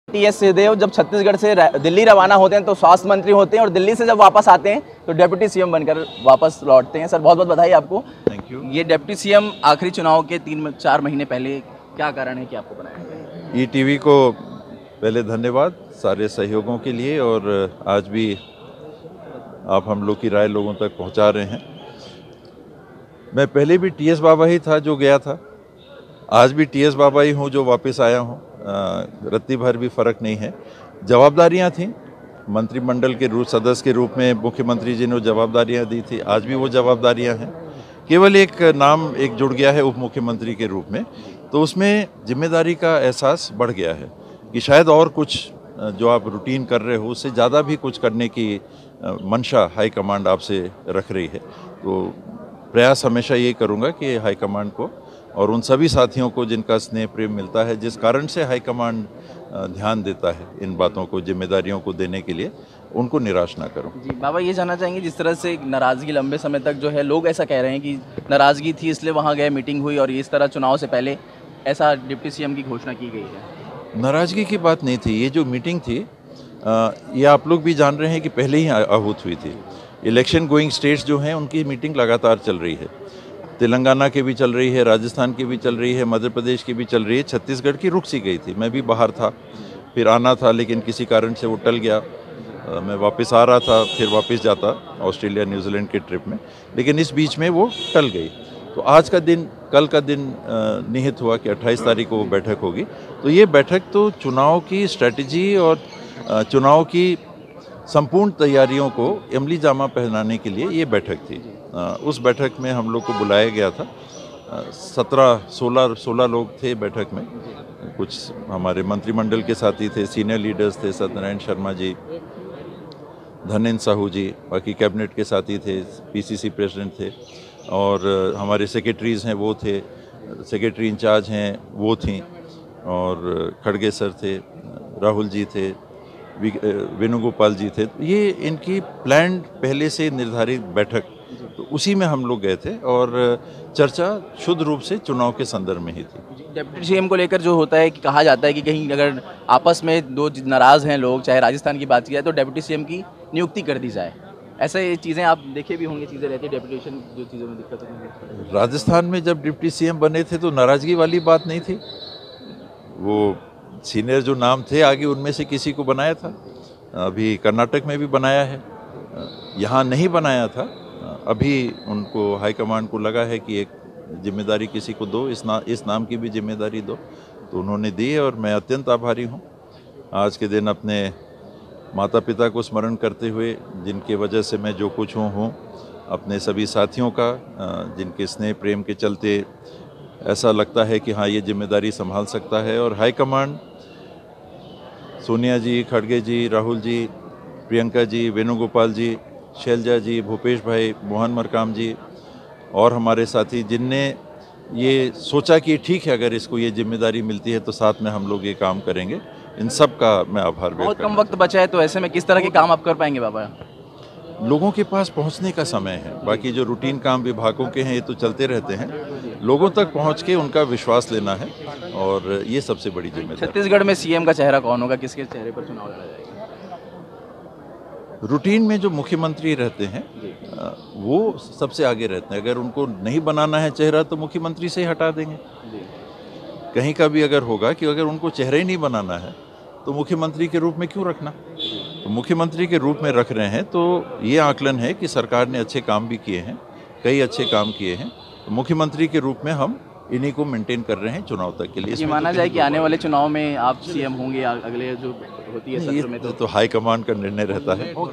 TS Singhdeo Interview : भूपेश बघेल ही होंगे सीएम फेस - टीएस सिंहदेव | Chhattisgarh Deputy CM
TS Singhdeo Exclusive Interview : स्वास्थ्य मंत्री और वरिष्ठ कांग्रेसी नेता टीएस सिंहदेव को Chhattisgarh का पहला डिप्टी सीएम बनाने का निर्णय लिया गया है. बुधवार को दिनभर चले बैठक के बाद हाइकमान द्वारा यह फैसला लिया गया है. गुरुवार को टीएस सिंहदेव दिल्ली से वापस राजधानी Raipur लौटे और सीएम फेस को लेकर पार्टी की स्थिति भी स्पष्ट की. साथ ही खबरों में सपोर्ट के लिए ईटीवी भारत का शुक्रिया भी अदा किया.